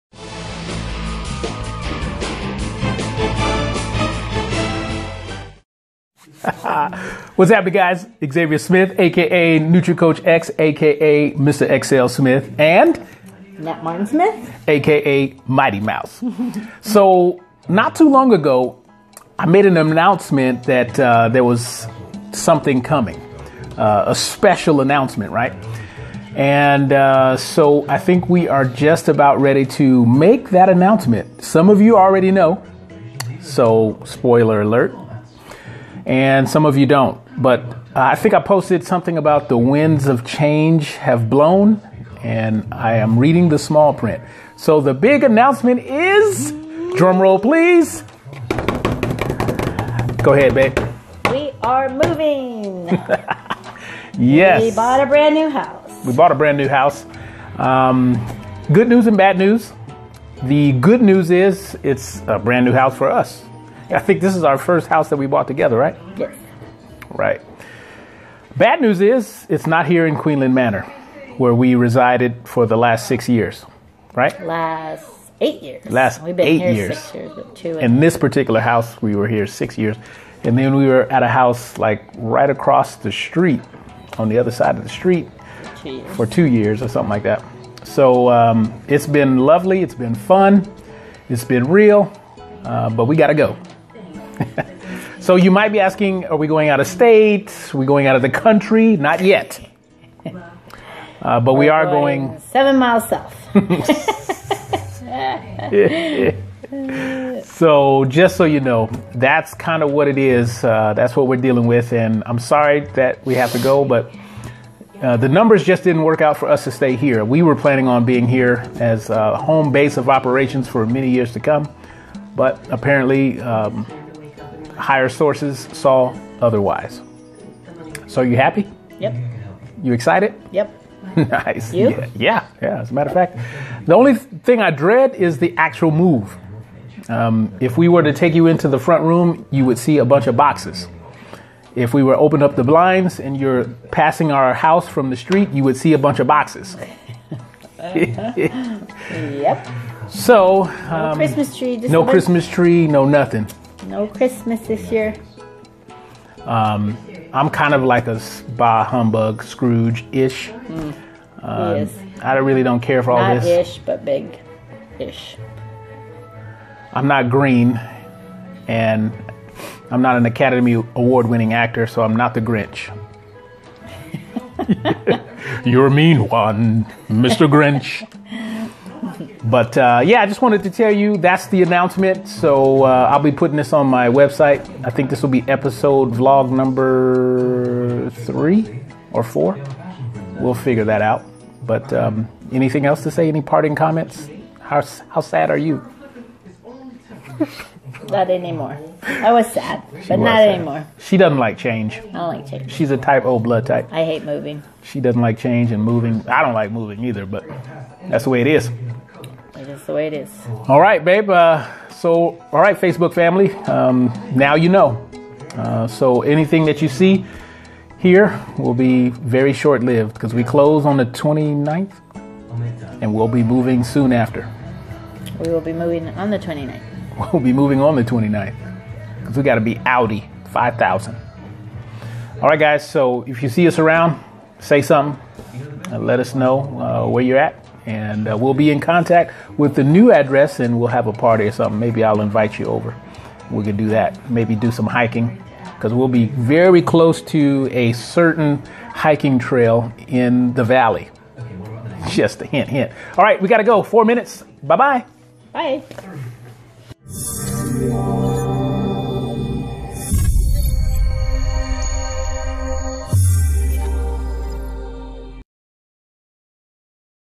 The Big Announcement - VLOG Episode #4
Queen Creek's highest-rated independent personal coach/trainer along with the valley's leading real estate re-developer Annette Martin-Smith is moving!